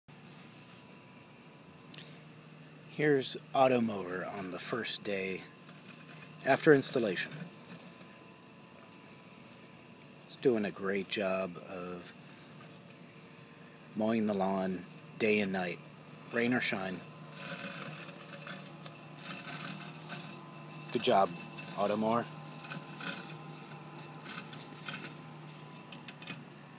Husqvarna Automower First Night Mowing Experience
This was our first night of mowing experience with the Husqvarna Automower.  Love it!  Taking care of business...